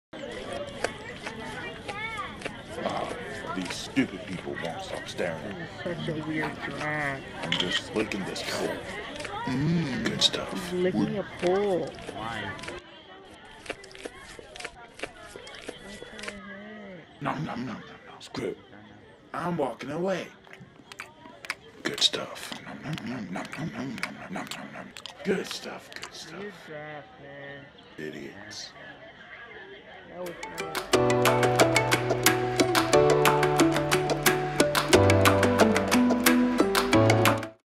THE STUPIDEST GIRAFFE EVER!?!
What the actual heck is this giraffe? This thing just stands there and licks the pole!! PLS HELP!!

I'm just a 14-year-old kid who enjoys making videos.

Credit:

Main Voice actor- Bobby E. Reidy

2nd Voice Actor-Andrew Temple